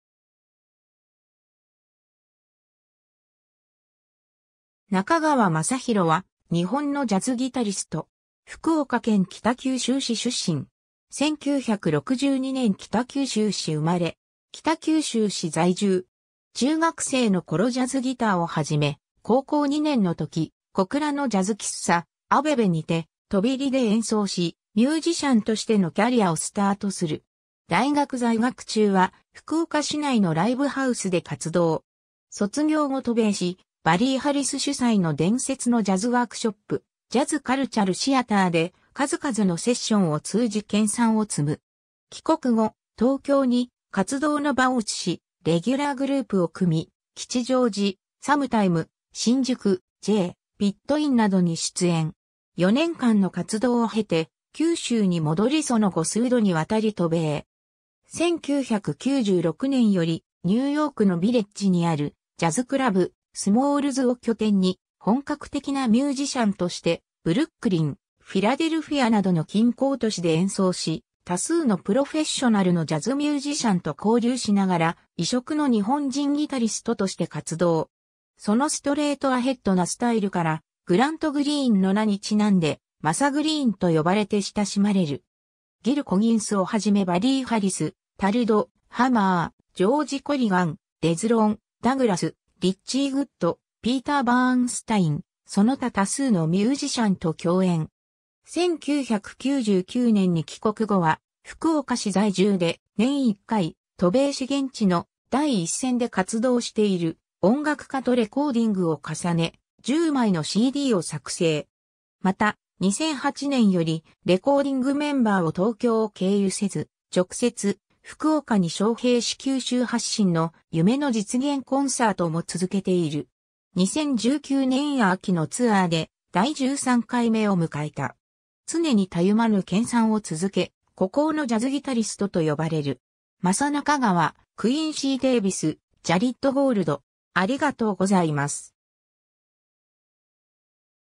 中川正浩
日本のジャズ・ギタリスト
中川 正浩（なかがわ まさひろ、Masa Nakagawa、1962年6月19日 - ）は、日本のジャズギタリスト。
福岡県北九州市出身。
 1962年北九州市生まれ。
北九州市在住。
 中学生の頃ジャズギターを始め、高校2年の時、小倉のジャズ喫茶「アベベ」にて飛び入りで演奏しミュージシャンとしてのキャリアをスタートする。
 大学在学中は福岡市内のライブハウスで活動。
卒業後渡米し、バリー・ハリス主宰の伝説のジャズ・ワークショップ「ジャズ・カルチャル・シアター」で数々のセッションを通じ研鑽を積む。
 帰国後、東京に活動の場を移しレギュラー・グループを組み、吉祥寺「SOMETIME」、新宿「J」、「ピットイン」などに出演。
4年間の活動をへて九州にもどりその後数度にわたり渡米。
 1996年よりニューヨークのビレッジにあるジャズクラブ「スモールズ」を拠点に、本格的なミュージシャンとしてブルックリン、フィラデルフィアなどの近郊都市で演奏し多数のプロフェッショナルのジャズミュージシャンと交流しながら、異色の日本人ギタリストとして活動。
 そのストレートアヘッドなスタイルからグラント・グリーンの名にちなんで"マサ・グリーン"と呼ばれて親しまれる。
ギル・コギンス(p)をはじめバリー・ハリス(p)、タルド・ハマー(p)、ジョージ・コリガン(p)、デズロン・ダグラス(b)、リッチー・グッド(b)、ピーター・バーンスタイン(g)、その他多数のミュージシャンと共演。
 1999年に帰国後は福岡市在住で年１回、渡米し現地の第一線で活動している音楽家とレコーディングを重ね、10枚のCDを作成。
また、2008年より、レコーディングメンバーを東京を経由せず、直接、福岡に招聘し九州発信の「夢の実現コンサート」も続けている。
2019年秋のツアーで第13回目を迎えた。
常にたゆまぬ研鑽を続け、孤高のジャズギタリストと呼ばれる。
 Masa Nakagawa,Quincy Davis,Jared Gold